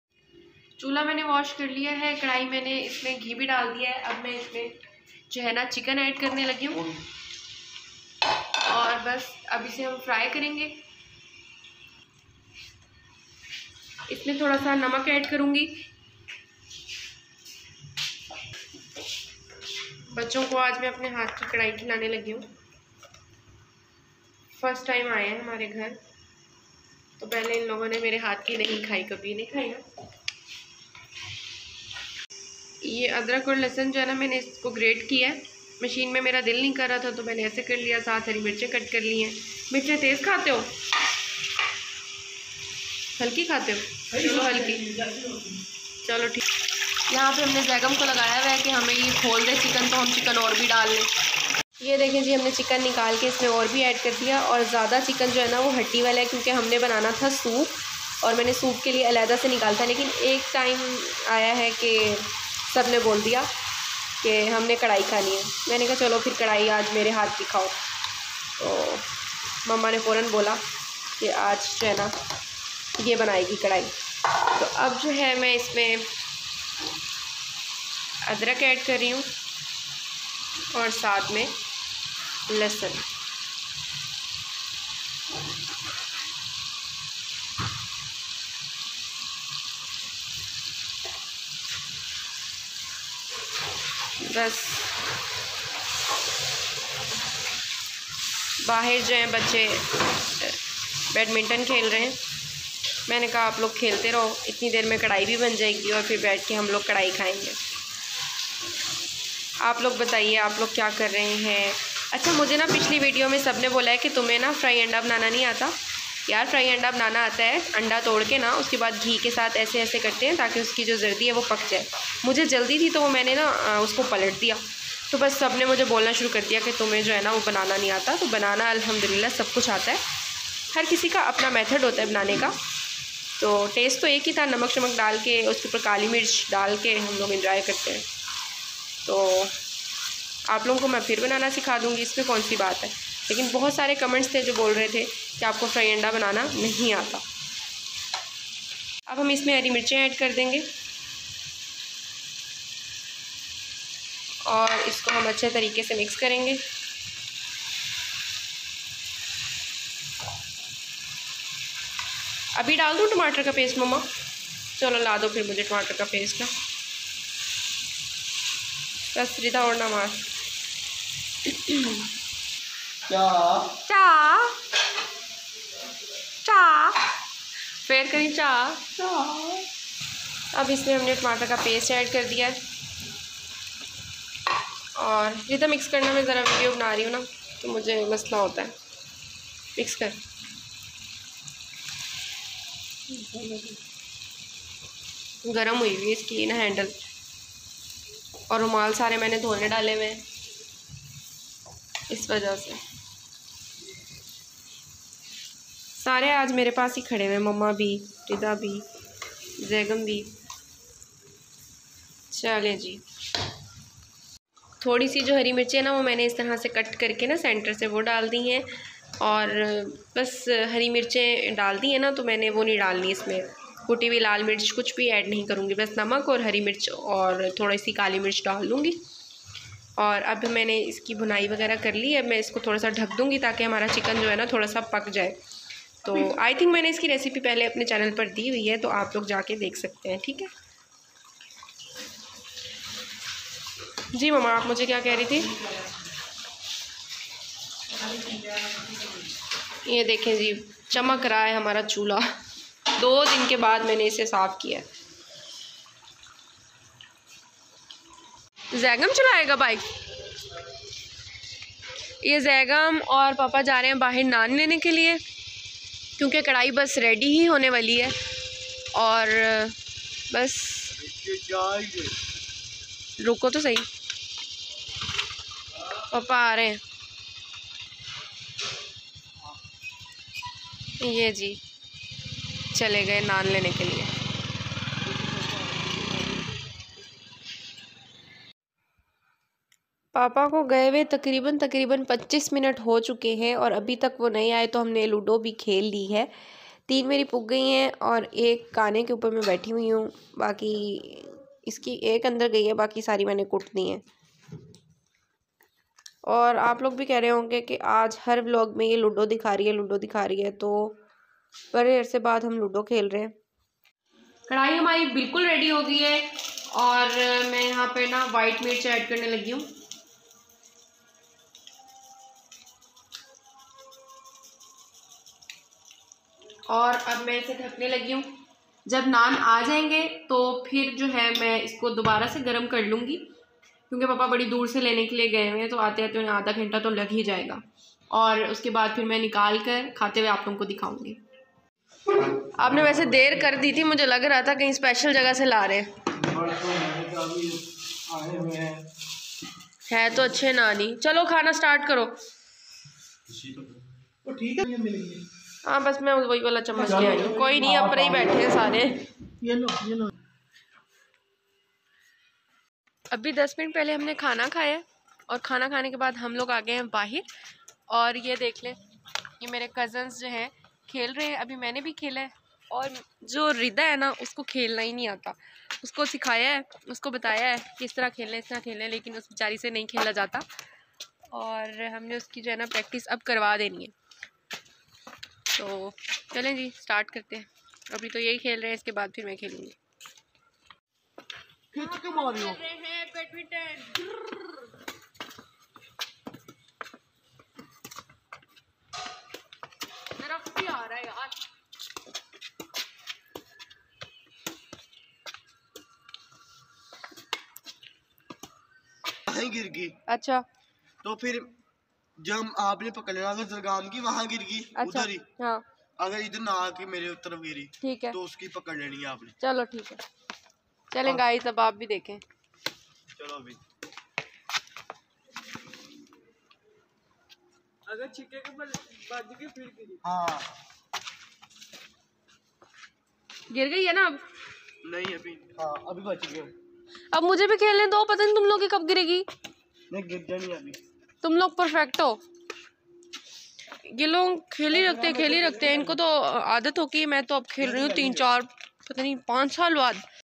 Raat Ke khane Mein Maine Banai chicken kadai - cousin se badminton khelna bhi Sikh liya
ummid hai ki meri aaj ki video aap Logon Ko acchi lagi Hogi Agar Meri video aap Logon Ko acchi lagi hai to ise like and share Jarur kariyega Aaj maine apne kitchen Mein chicken kadai Banai Thi Aur Raat Ke khane Mein Humne usko Nan ke sath Sarv Kiya aur bahut hi enjoy Kiya agar aap logon ko meri Sky ki recipe acchi Lage to mujhe feedback bhi dijiyega aur meri video ko like bhi kariyega khana khane ke bad Ham chale gaye the Sab Bahar aur Bahar jakar Humne badminton ke alava aur bahut bahut maja Aaya